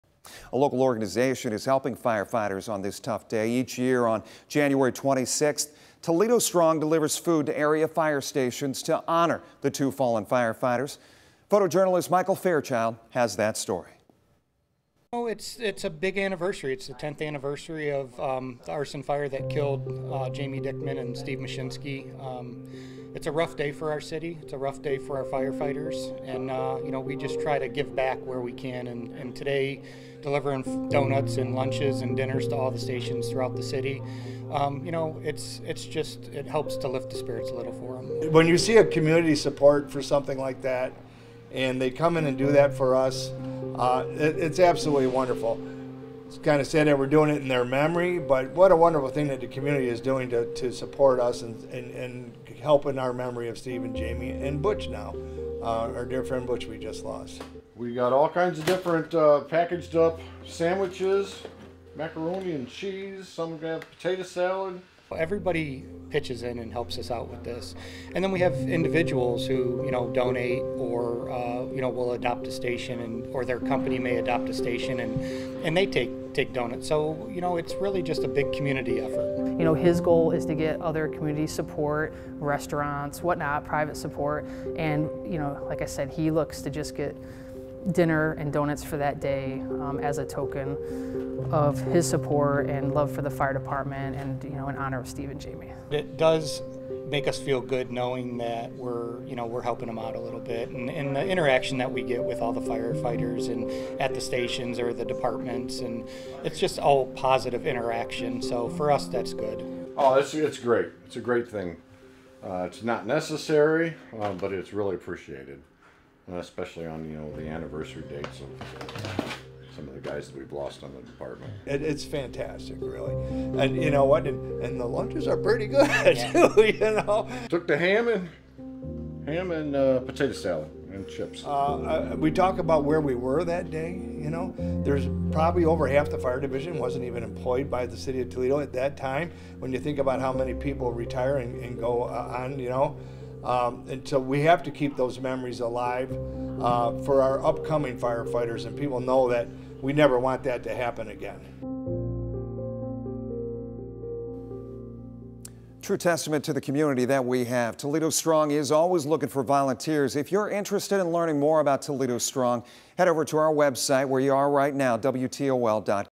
Toledo Strong honors fallen firefighters with food delivery to Toledo firefighters
Each year on Jan. 26, the group delivers food to Toledo fire stations to honor the loss of firefighters killed on this date in 2014.